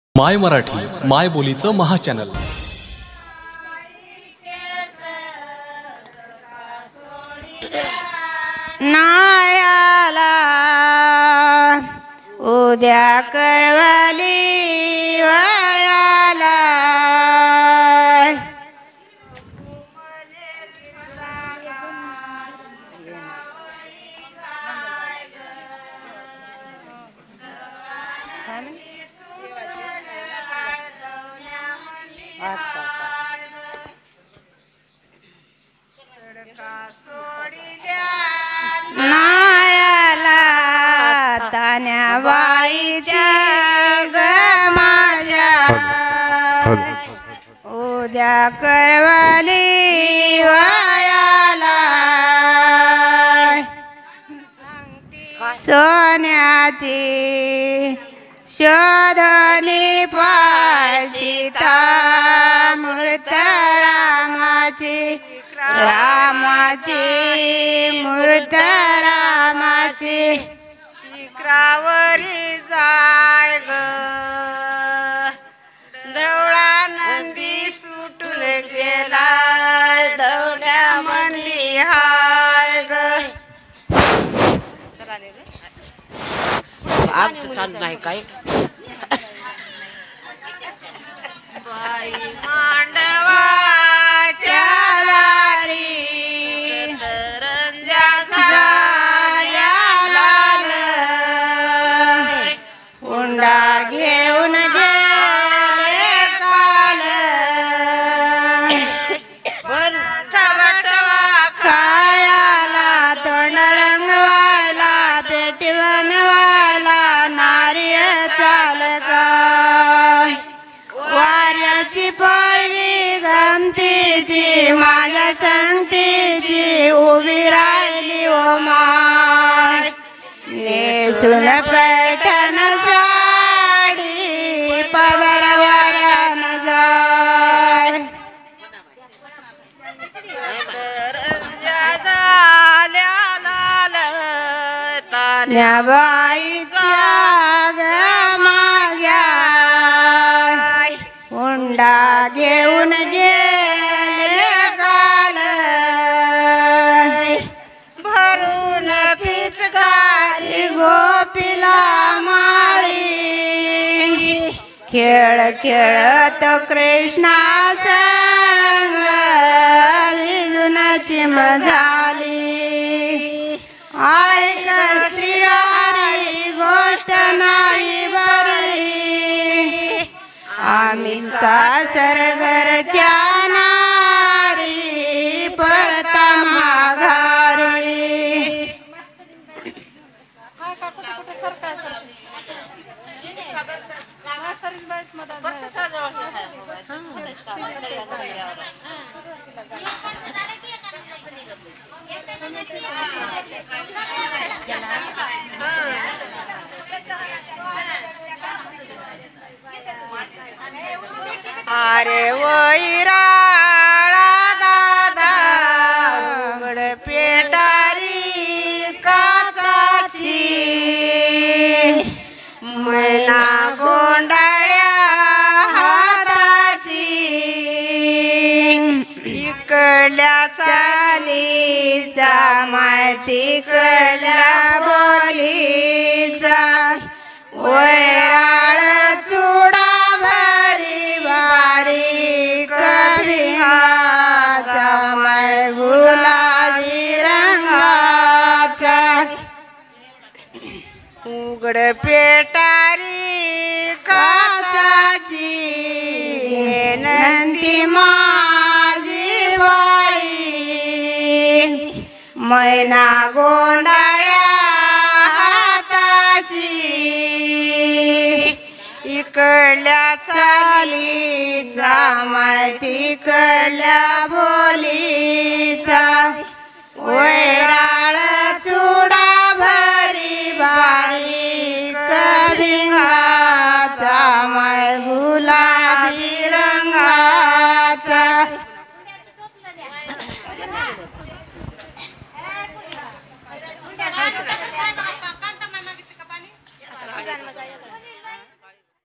शिदोरी(रुखवत)करताना गायलेली गाणी,लोकगीत,मराठी गीत,folk song,हळदीचे गाणे, haldi song,jatyavchya ovya,
मराठी विवाह.. विवाह जरी एक दिवसात उरकत असला तरी त्यापूर्वी जवळपास महिनाभर वेगवेगळ्या कार्यक्रमांची रेलचेल असते.विवाहाच्या दोन दिवसांपूर्वी गावातील बायकांना बोलावून शिदोरी(रुखवत) तयार केला जातो.यावेळी जमलेल्या बायका विविध दुर्मिळ गाणी गातात.
नांदेडच्या कंधार तालुक्यातील महालिंगी येथील महिलांनी अशाच कार्यक्रमात गायलेली ही सुंदर लोकगीतं आणि ओव्या खास आपल्यासाठी.

'माय मराठी '.
मायबोलीचं महाचॅनल. !!

mai Marathi...
the biggest channel of the mothertoung.
-rajesaheb kadam